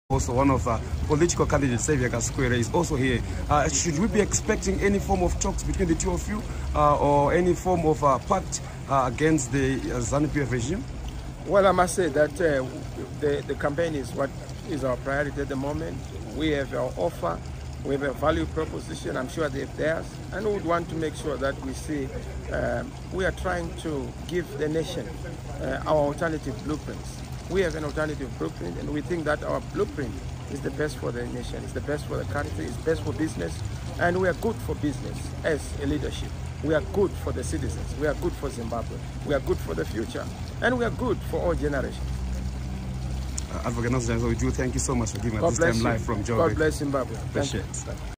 Chamisa Dismisses A Pact With Kasukuwere | 2023 Elections
Today In Johannesburg South Africa I interviewed the @ccczimbabwe leader  @nelsonchamisa wether he will be engaging @Hon_Kasukuwere on possible political pact.